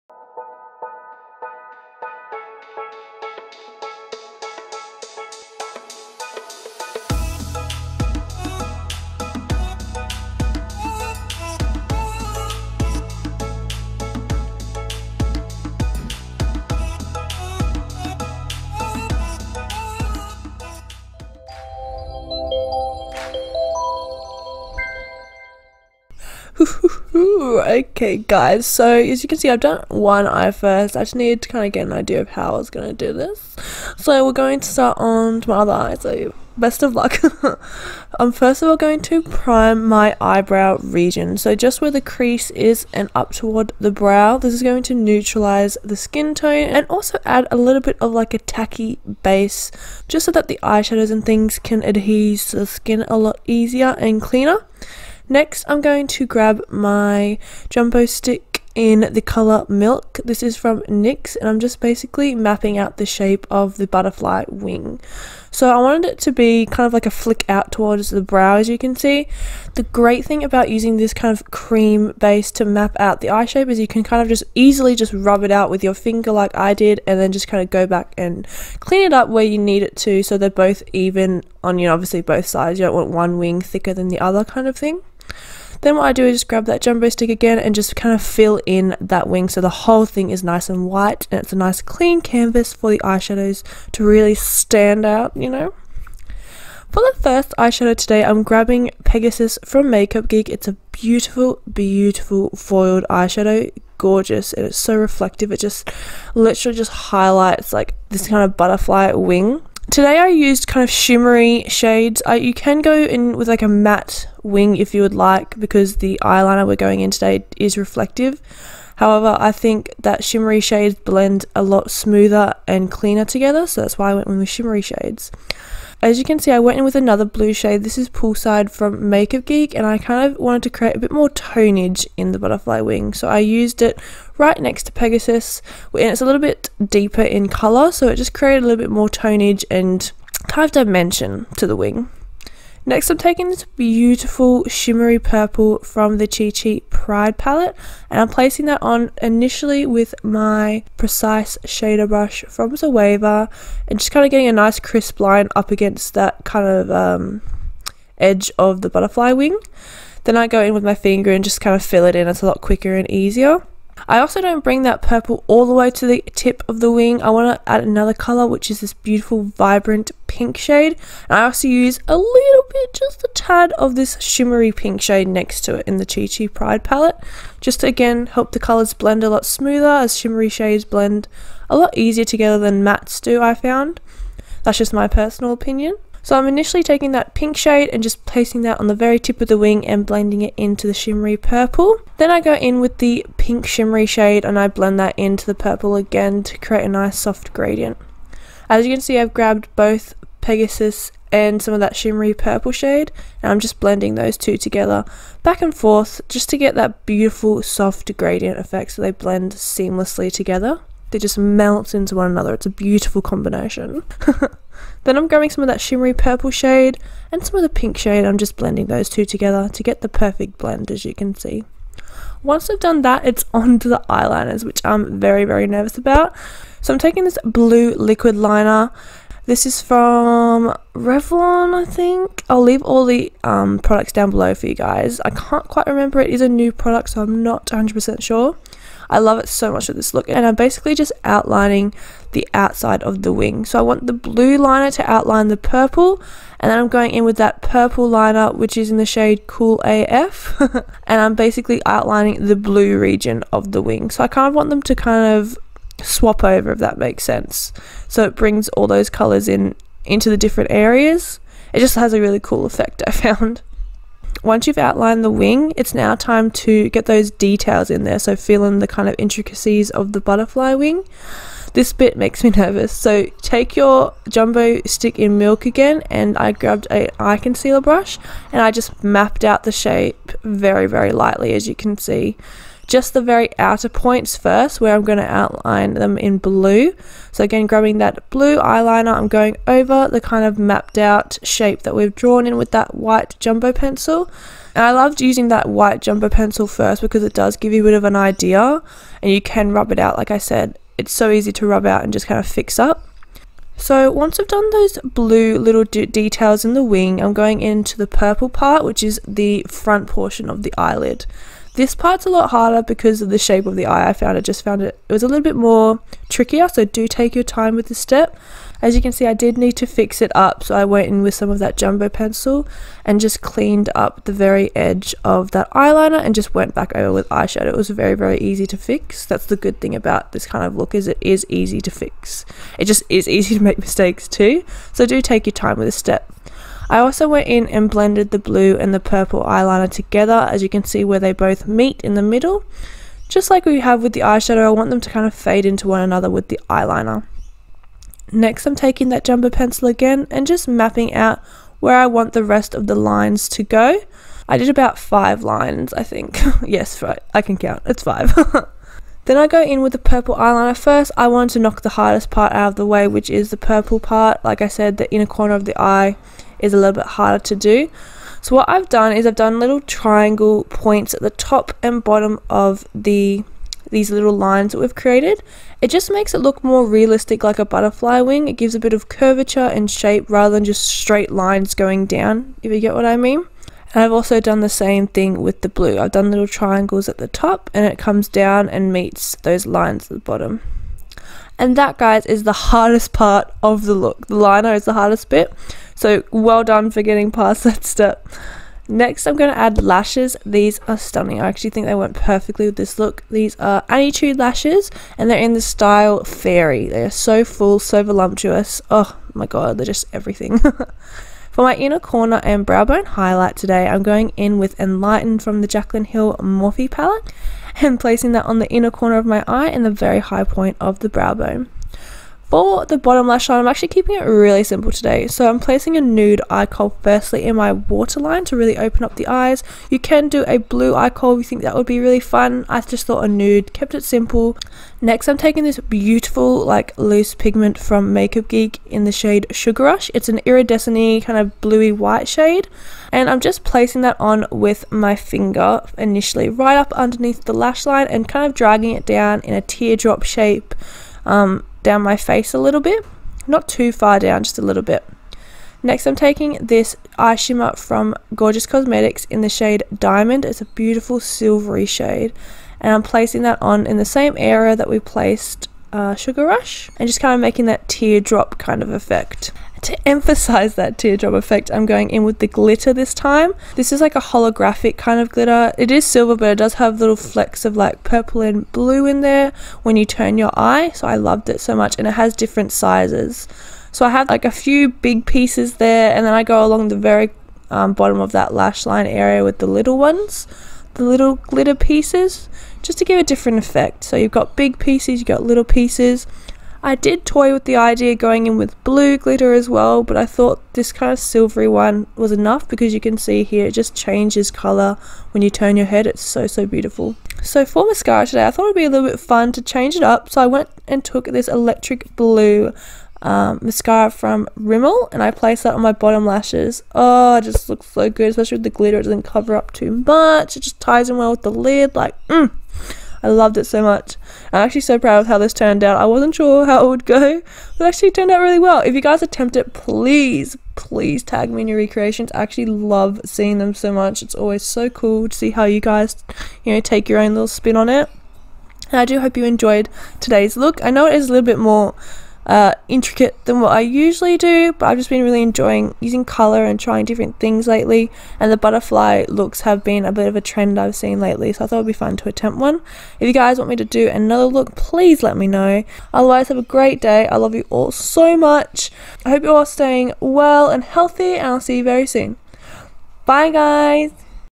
Butterfly Eyes Makeup Tutorial 🦋
I've been LOVING the butterfly makeup trend lately and thought I'd do my own little spin on it.
Enjoy my loves xx

LET’S CONNECT!